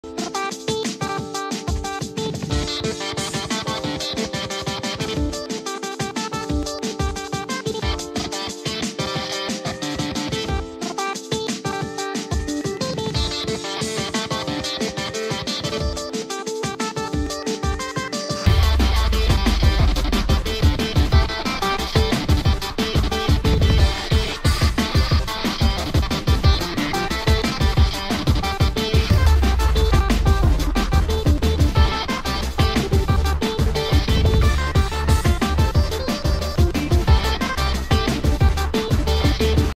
Whitty VS Boyfriend Fire Fight In Real Life (FNF IRL)
BF Challenges Whitty to a fight between flame-throwers IRL
VS Fire Whitty (Whitty Fire Mod) is a Friday Night Funkin’ Mod inspired by Chino’s animation
Thanks to ._mazii__ for participating in the video